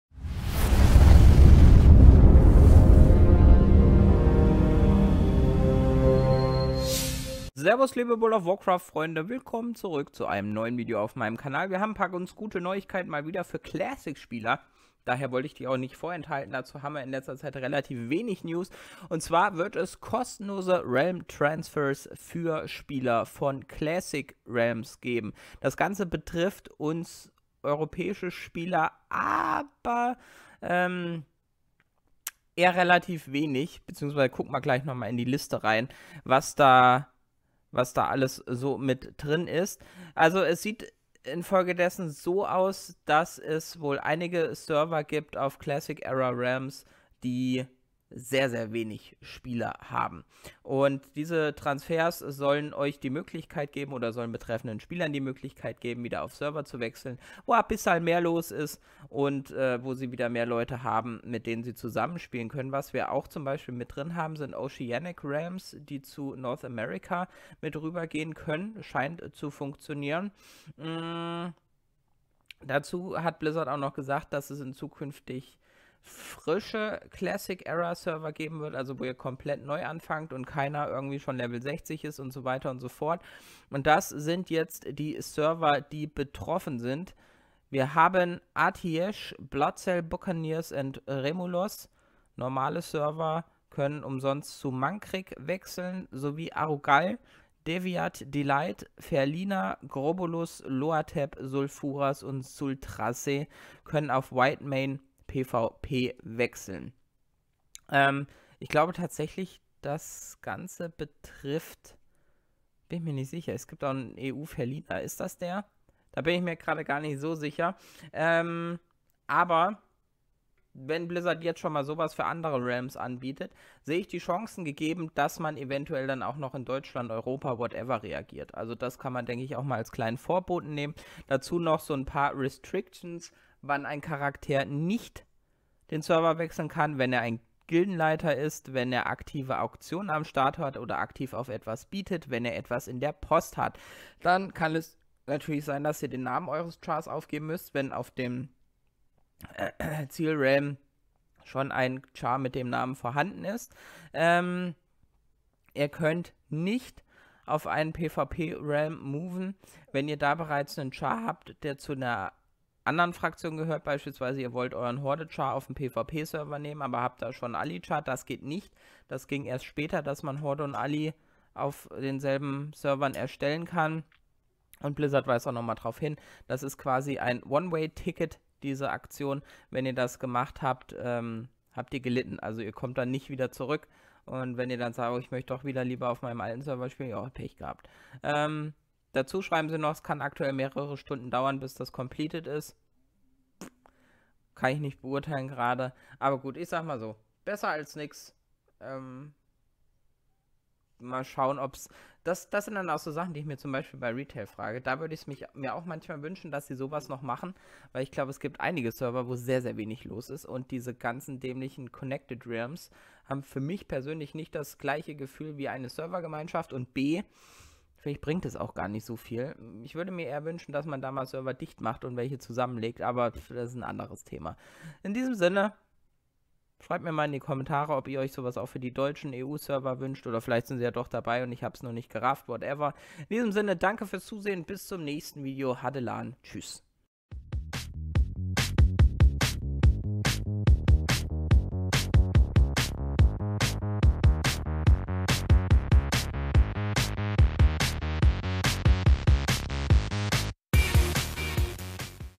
World of Warcraft Classic: Blizzard bietet kostenlose Realmtransfers an - aber nicht für alle
Ausgewählte Spieler dürfen ihren leeren Classic-Server kostenlos verlassen und auf einen volleren Server wechseln. Was haltet ihr von der Aktion?